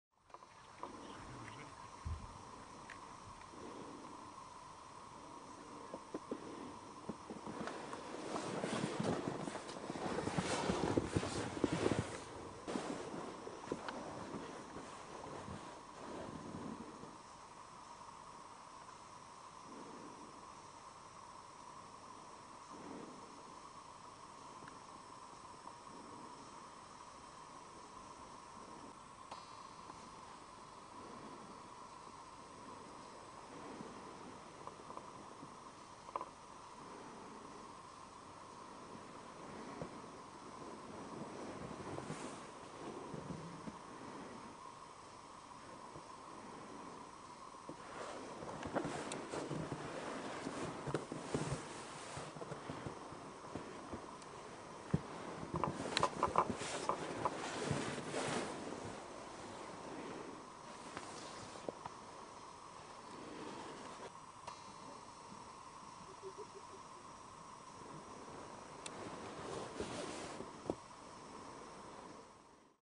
The Banished Dragon, Ice Hotel, Sweden 2008
The Banished Dragon ice suite at the Ice Hotel in Kiruna 2008.  One of the best designed rooms!